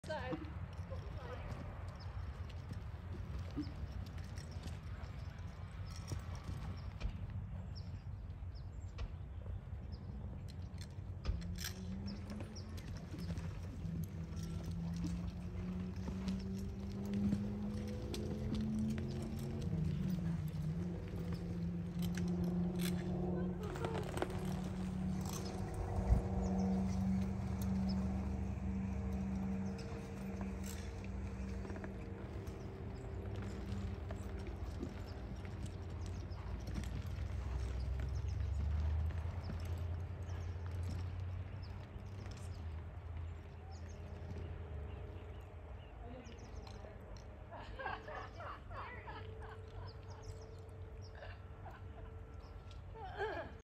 Pac Man Cat – round pen
EXCELLENT Open, Youth or Non Pro horse
Pac Man Cat – $60,000
2018 AQHA sorrel gelding

Pac Man Cat is super fun on a cow. He is the most versatile, athletic horse. He loves to compete or just head out on the trail. Pac Man with limited showing, has earnings of just over $5k.